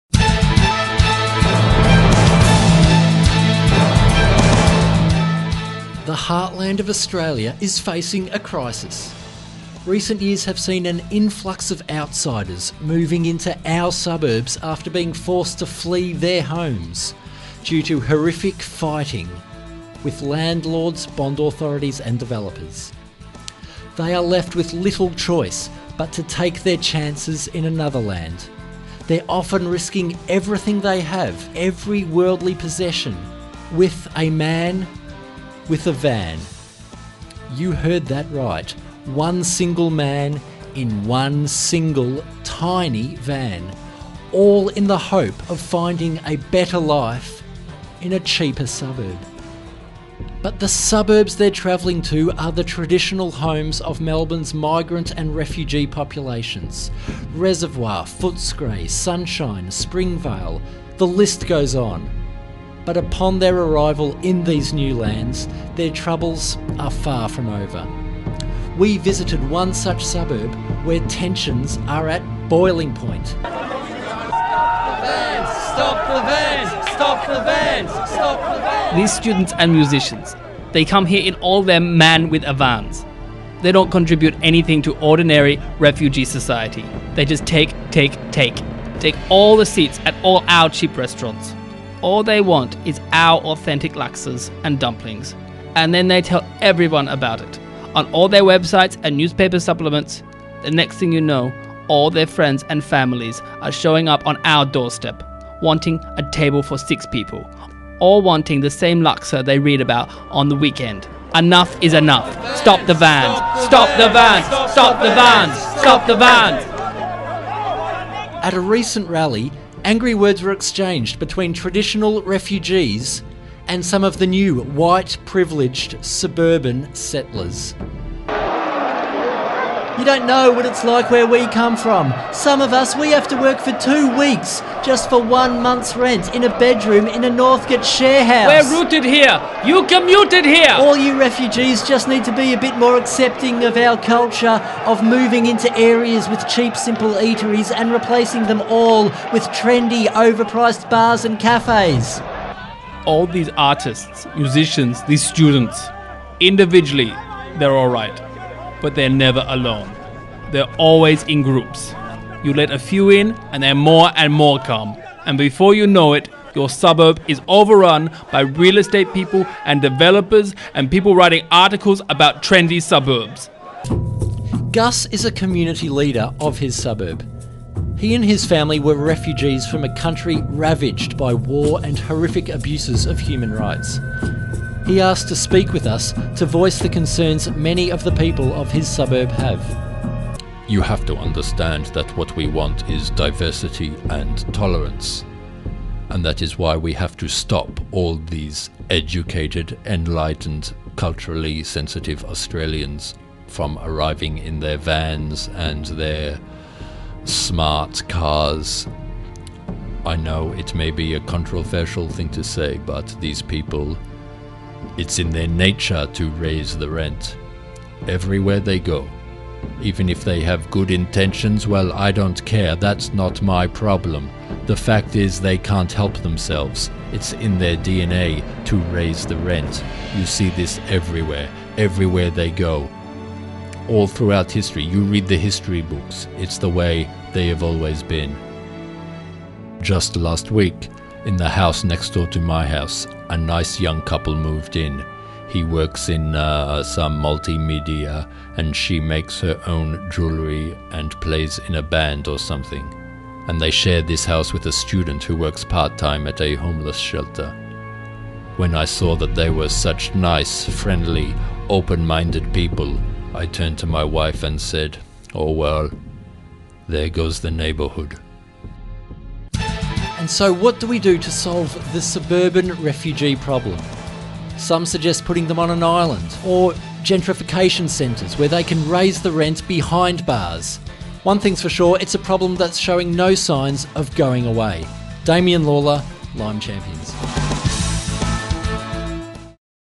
LIME CHAMPIONS Suburban Refugees
A report about the influx of white, inner city dwellers into the poorer, more culturally diverse suburbs which are the traditional home to Melbourne's refugee population.